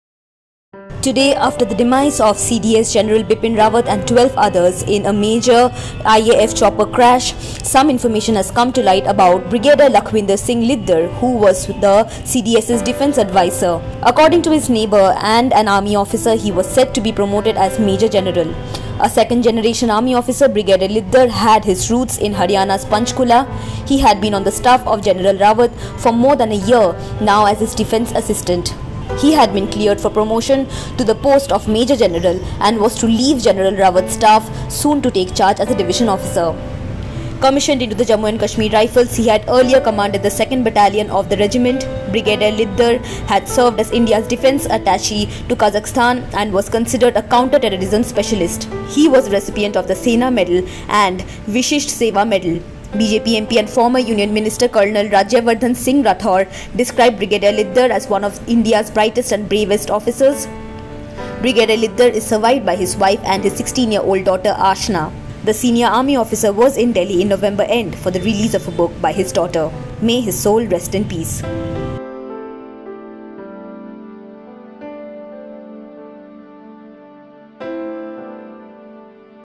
Brigadier LS Lidder, defence assistant to General Rawat, was ‘set for a promotion’ | Oneindia News
Know all about Brigadier Lakhwinder Singh Lidder who was in the MI-17V5 chopper with CDS General Bipin Rawat.